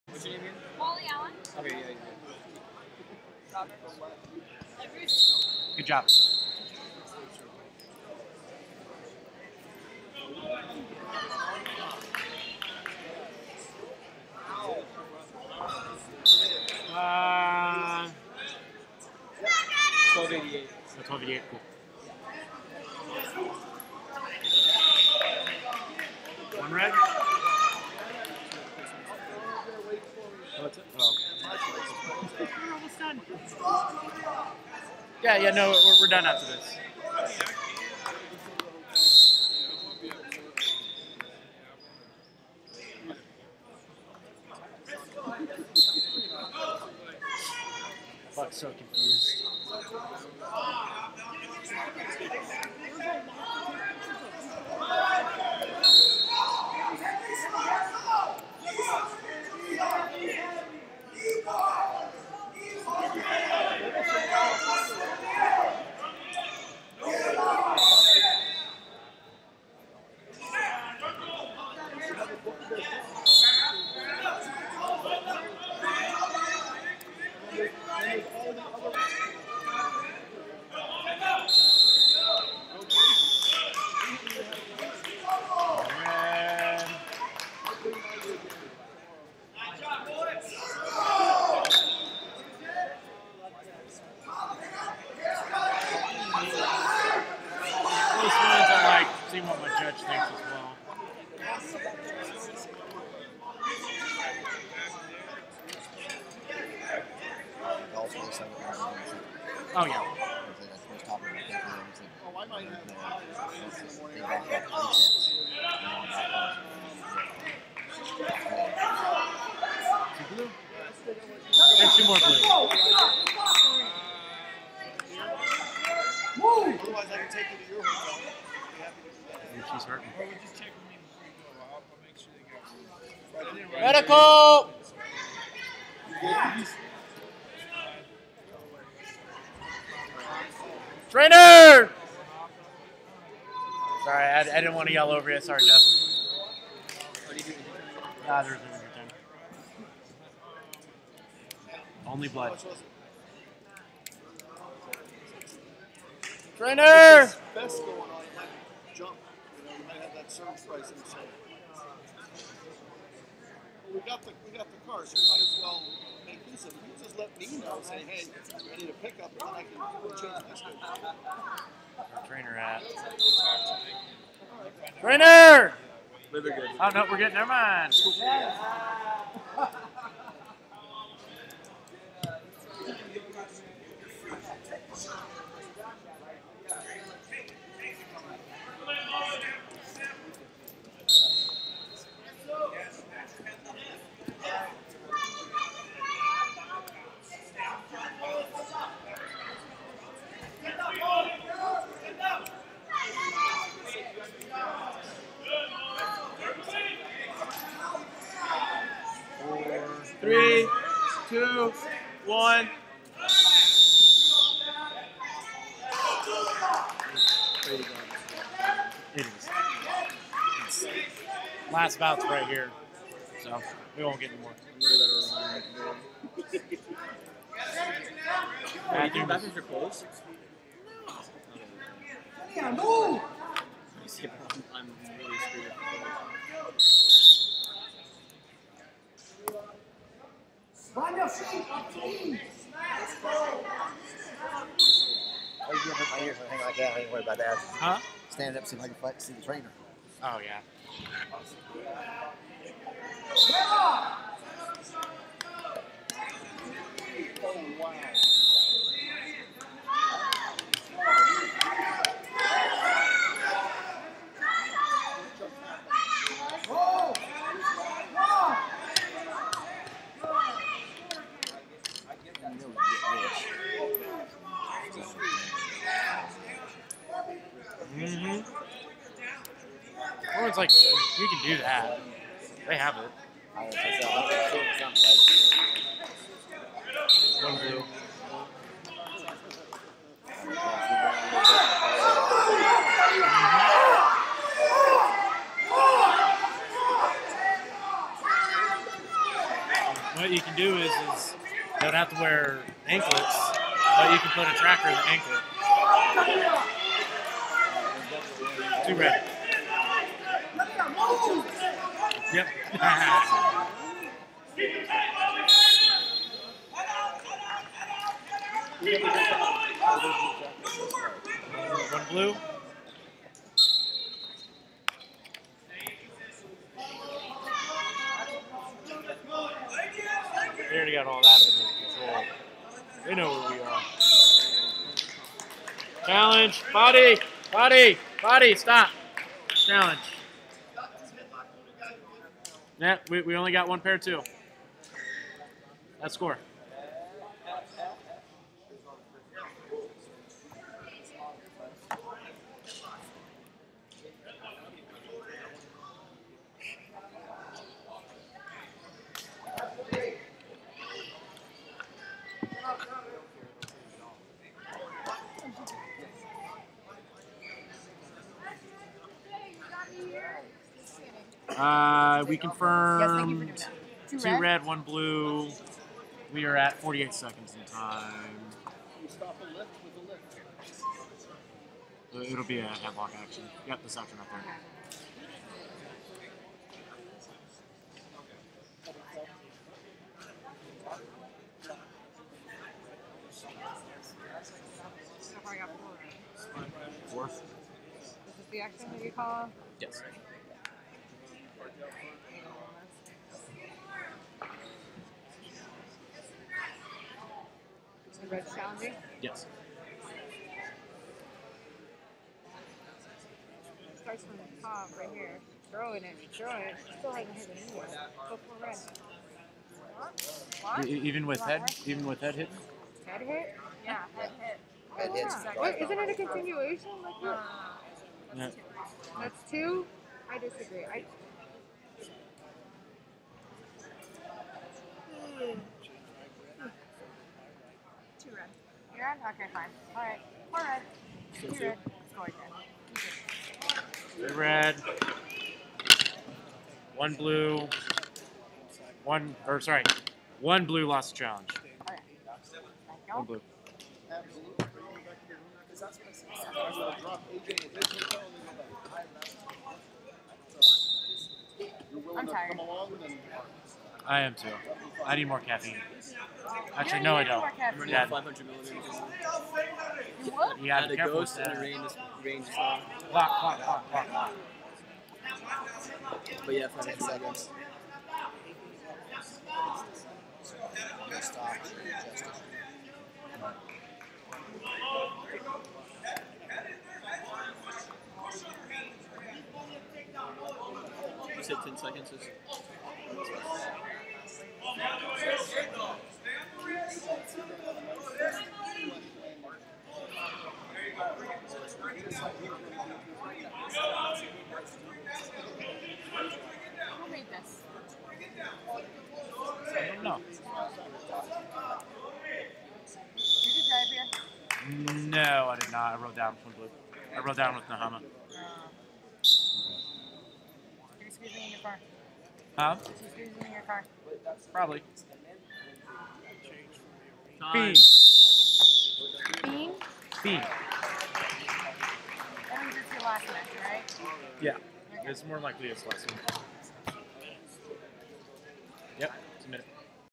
50 Kg Consolation - Molly Allen, The Best Wrestler Vs Libby Roberts, Inland Northwest Wrestling Tr
2022 USMC Women's National Championship, UWW U15, Cadets, Jr & U23 WTT - 50 Kg Consolation - Molly Allen, The Best Wrestler Vs Libby Roberts, Inland Northwest Wrestling Training Center 6461